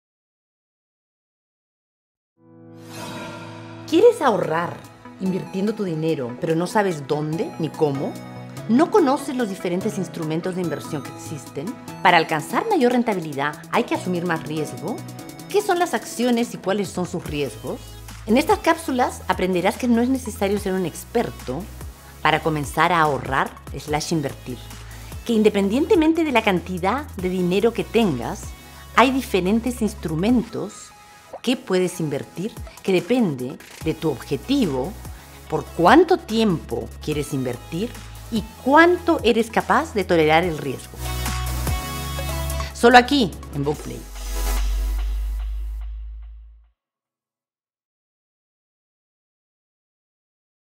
Comenzando a ahorrar e invertir
En este curso aprenderás sobre los diferentes instrumentos para invertir y ahorrar, la variación de los riesgo y la rentabilidad asociada.